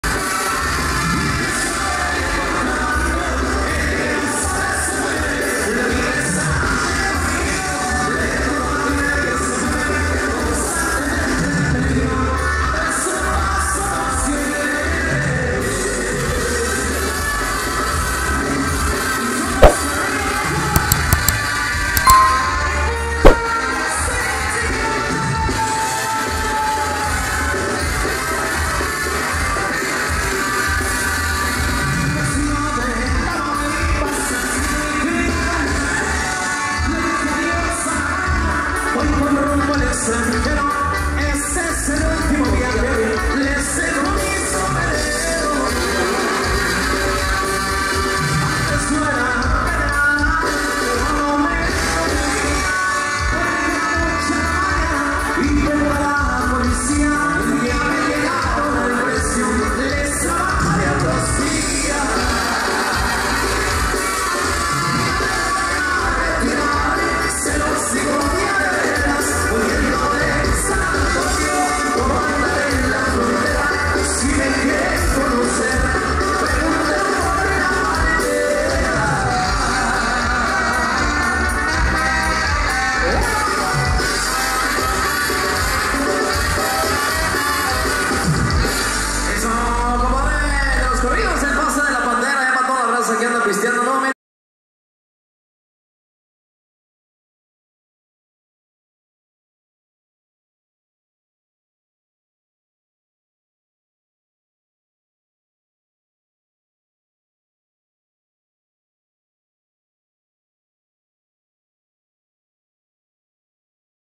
OMG pero que BELLEZA esa chica de vestidito AZUL en este bailazo 😍
Bienvenidos a mi canal gente bonita aqui les dejo este bailazo en Zacapuato, Guerrero.💃🔥🎀😍📹🎞